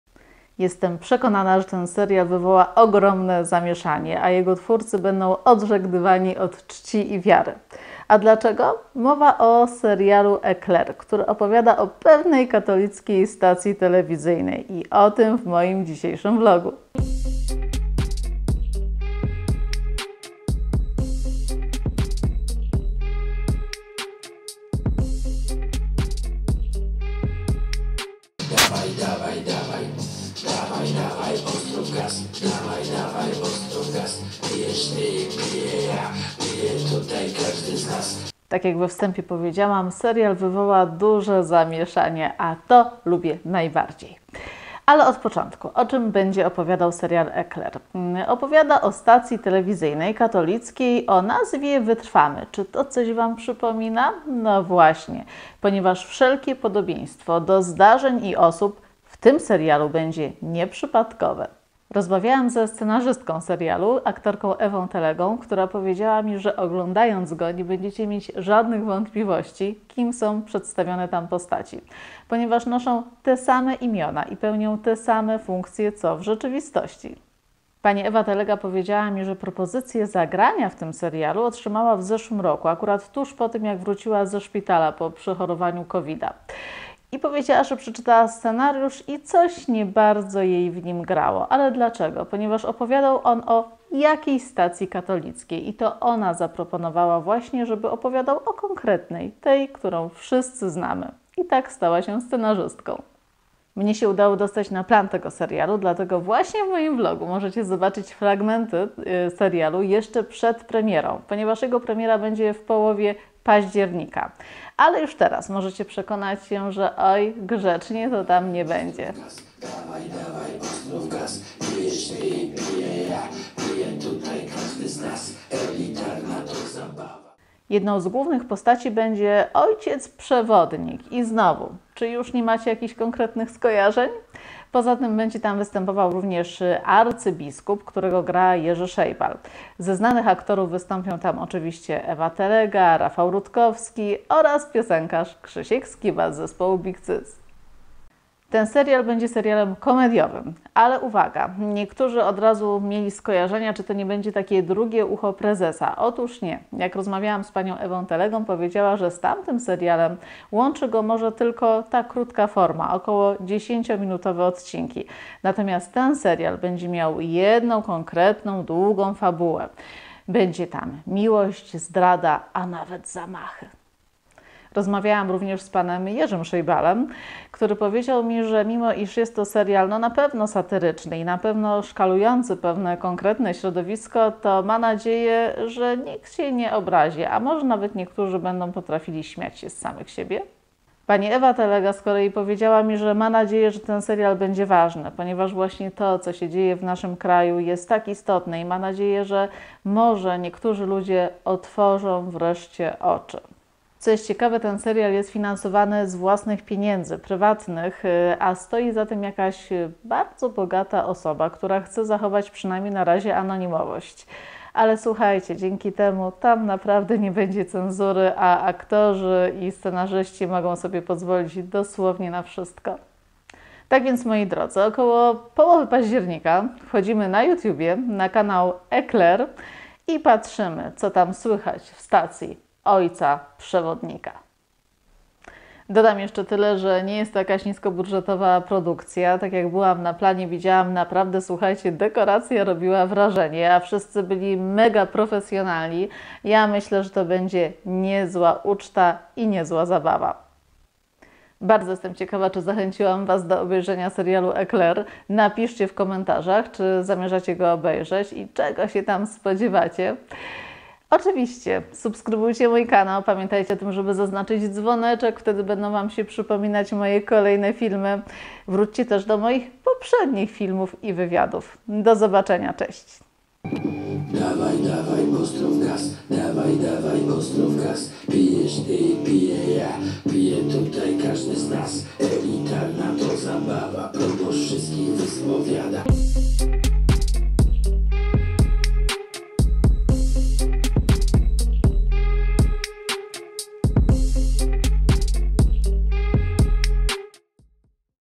Ekler - serial, który oburzy PiS, Kościół i Rydzyka. Pierwsze zdjęcia z planu. Bez cenzury ***** ***
W połowie października na YouTube premiera serialu "Ekler". Będzie on opowiadał o katolickiej stacji telewizyjnej Wytrwamy, którą zarządza Ojciec Przewodnik. Wszelkie podobieństwa do osób i zdarzeń są NIEPRZYPADKOWE.

Dzięki Tobie będę mogła dalej rozwijać ten kanał.

►Dziękuję za obejrzenie tego filmu. Czekam również na Twój komentarz pod filmem